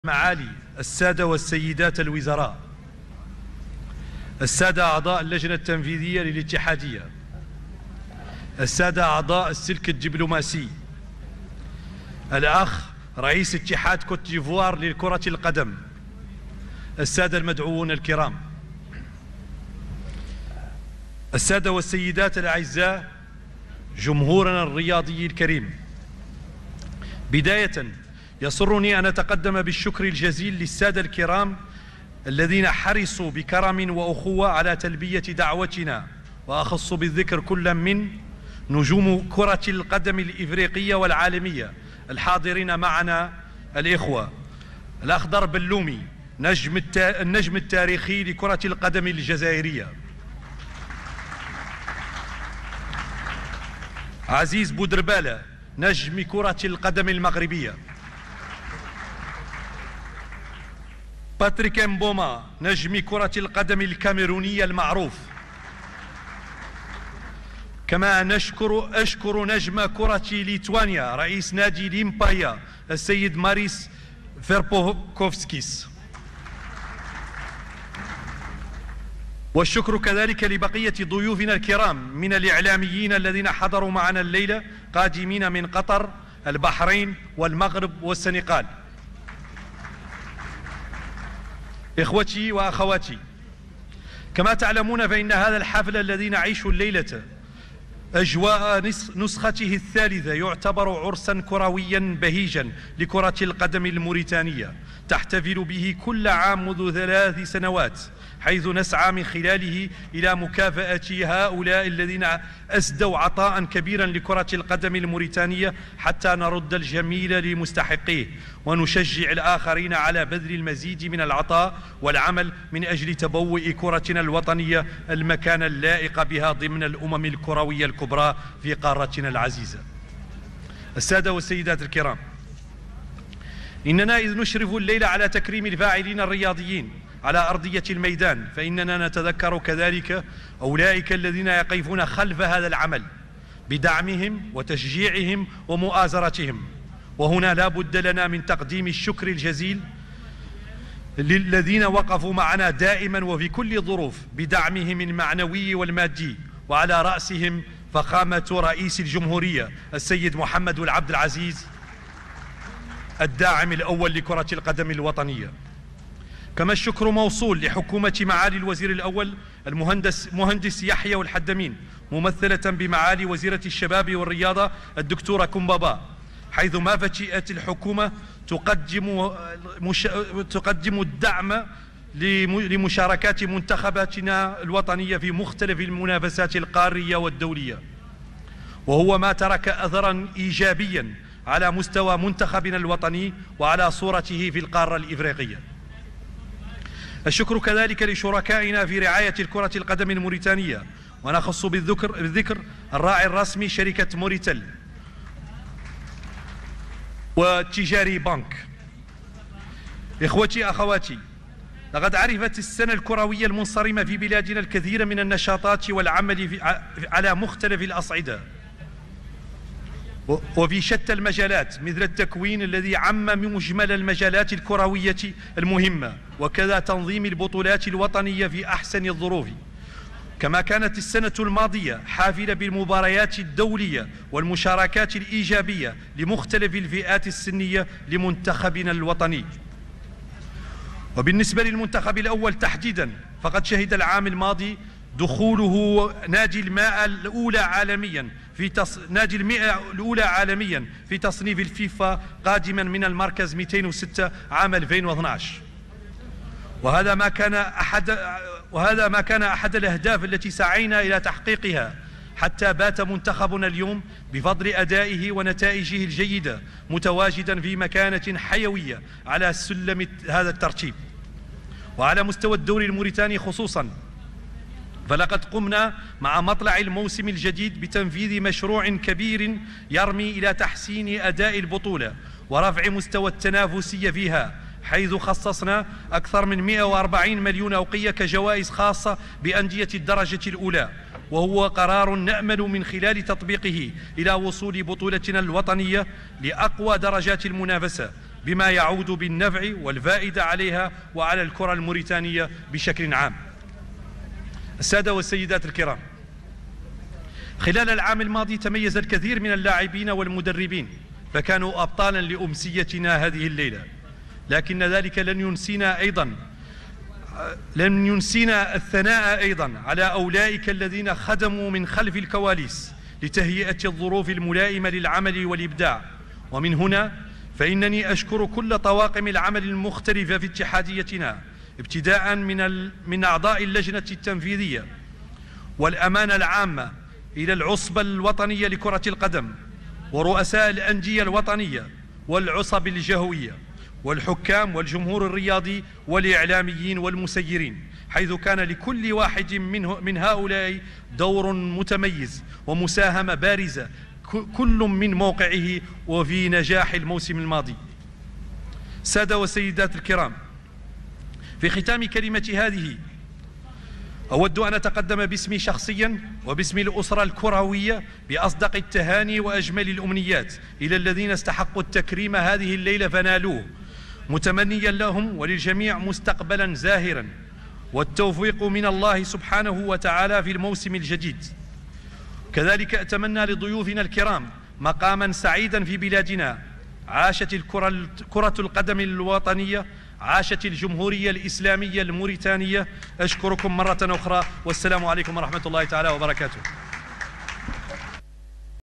كلمة رئيس الاتحادية الوطنية لكرة القدم السيد أحمد ولد يحي في حفل FFRIM Awards 2016
FFRIM Awards 2016 live